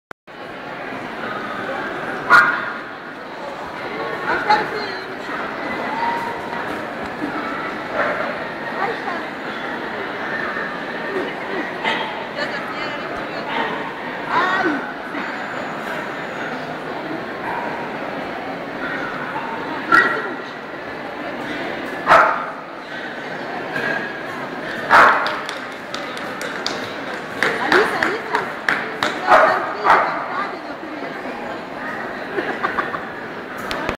CH Karmino Made Improvisation winning best female at Vilnius national dog show
Irish setter ring in Vilnius national dog show on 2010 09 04. Karmino Made Improvisation "Milly" wins best female.